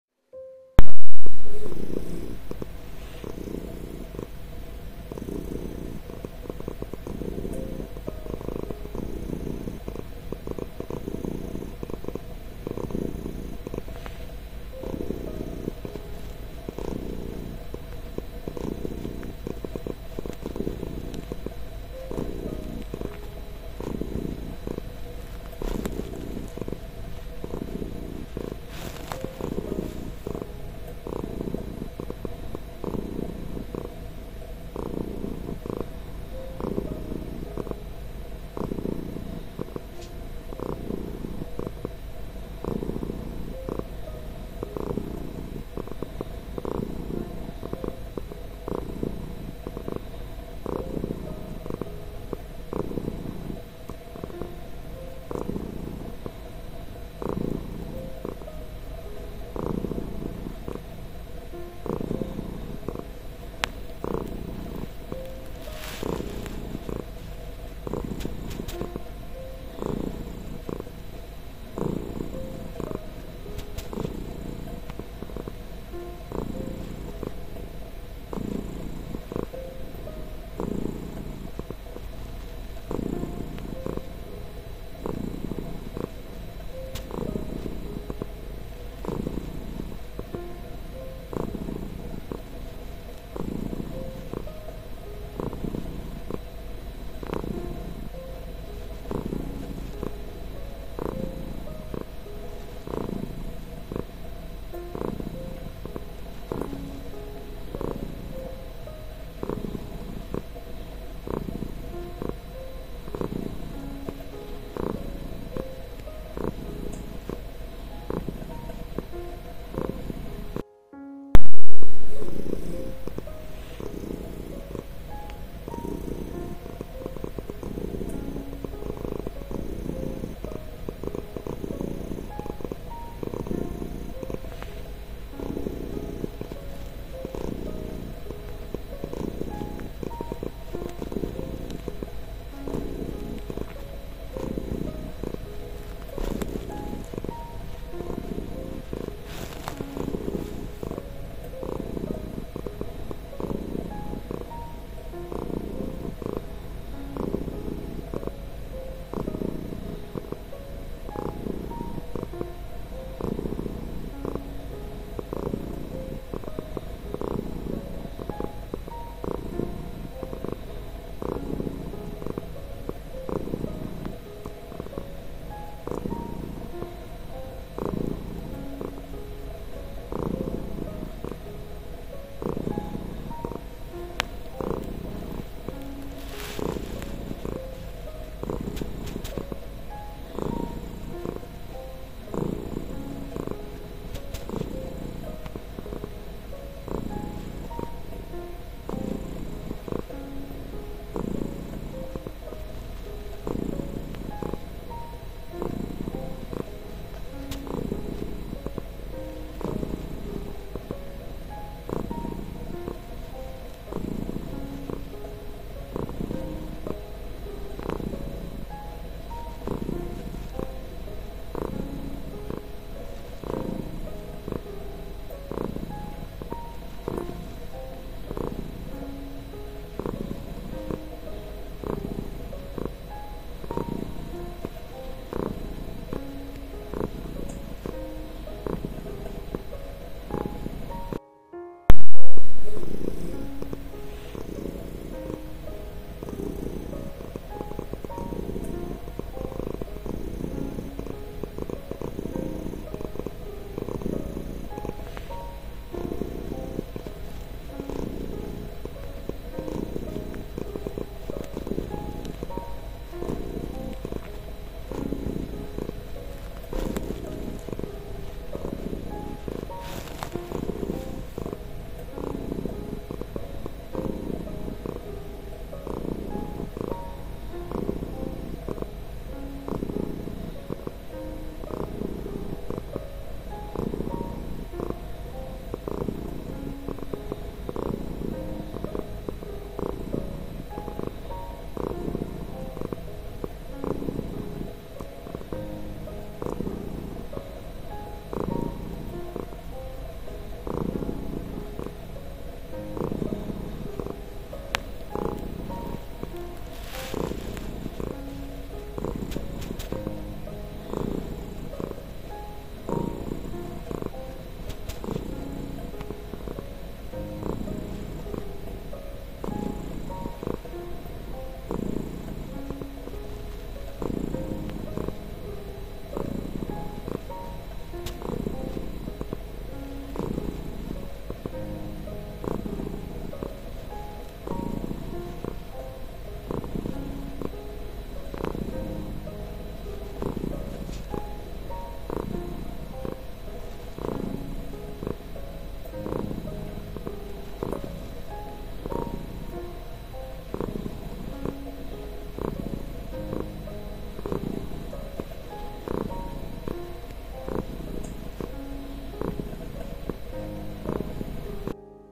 ASMR Cute Tricolor Kitten Girl Sleeps to Pleasant Music And Purrs
The kitten has played enough, is tired and is sleeping a sound sweet sleep. And what is he dreaming about?